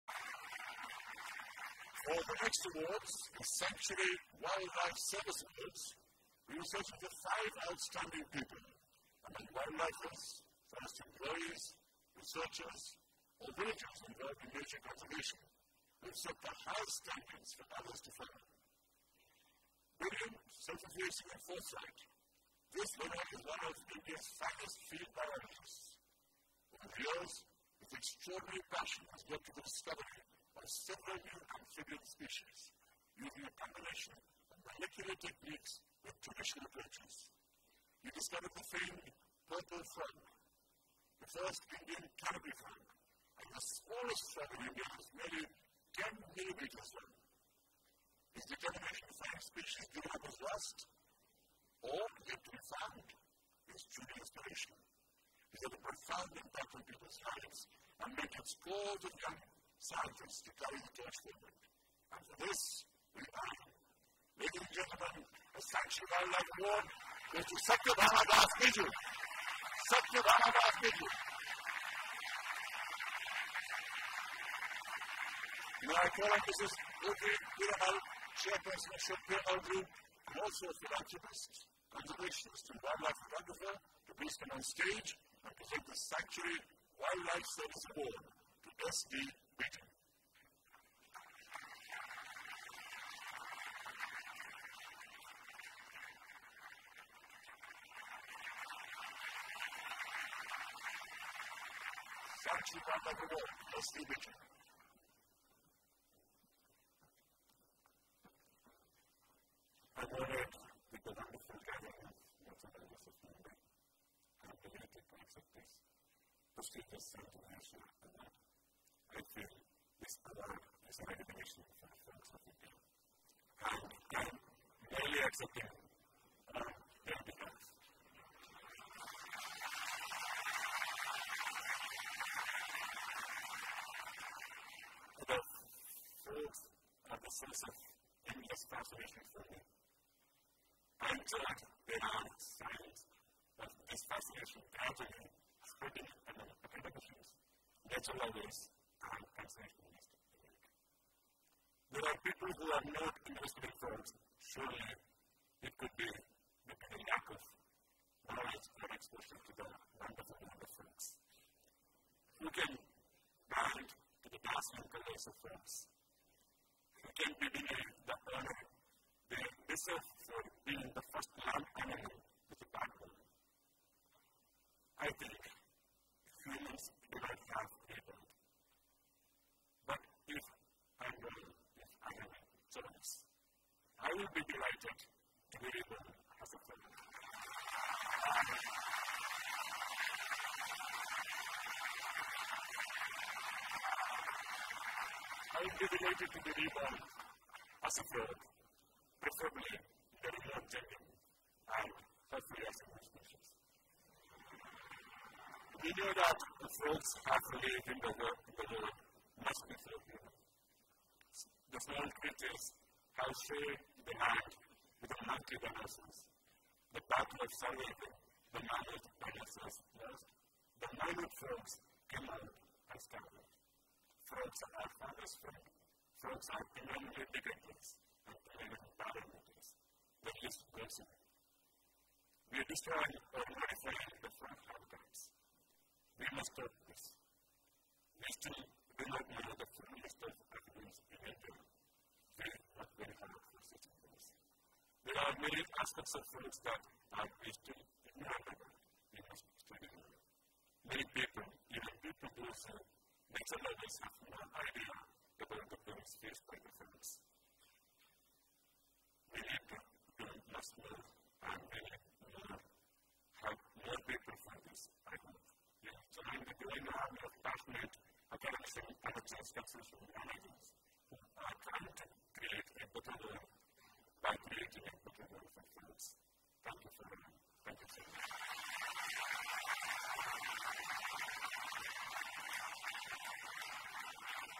SD Biju: Sanctuary Asia Wildlife Service Award 2011
SD Biju receives Sanctuary Asia Wildlife Service Award 2011 for Amphibian conservation and research.